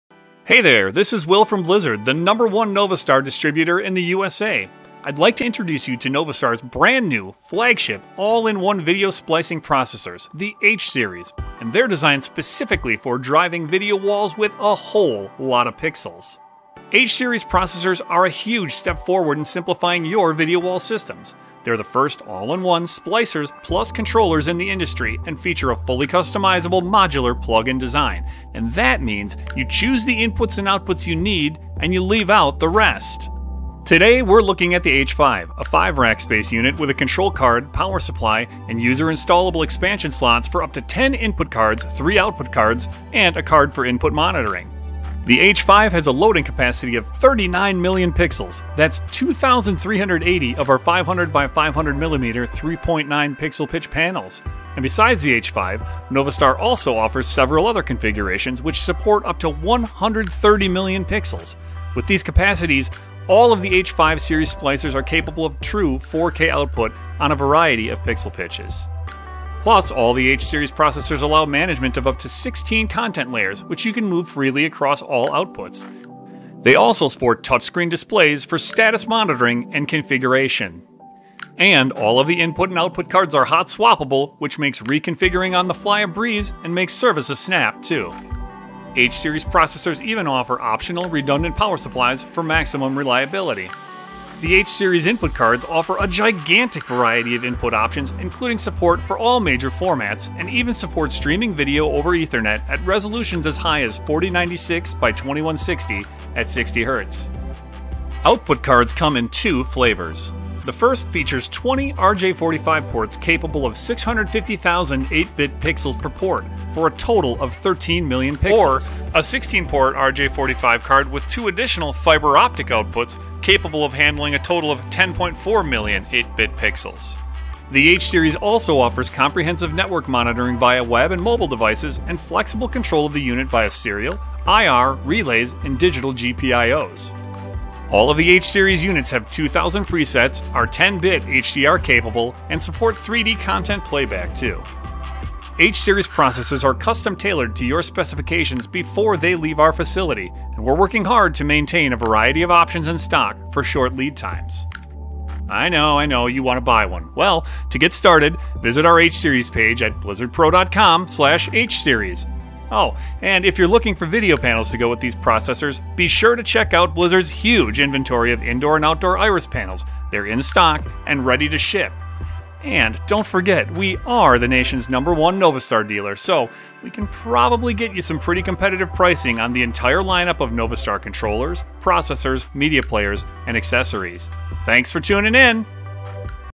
NovaStar™ H Series All-in-One Video Splicing Processors [PRODUCT OVERVIEW]
H Series is NovaStar’s flagship all-in-one video splicing processor, designed specifically for fine-pitch LED applications. It is the first all-in-one splicer and controller in the industry, which completely simplifies systems integration.

Blizzard Lighting is the #1 U.S. distributor of NovaStar LED video controllers & processors, and presents an overview on the H5, which is one of four processors in the H Series.

H5 is built with a five-rack chassis and has a maximum loading capacity of 39 million pixels + true 4K video processing.

H Series video splicing processors have a modular design & are custom-built units to fit specific application needs. And with the leading image processing technology in the industry, H series gives an astonishing visual effect, truly making it the perfect solution for fine-pitch LED applications.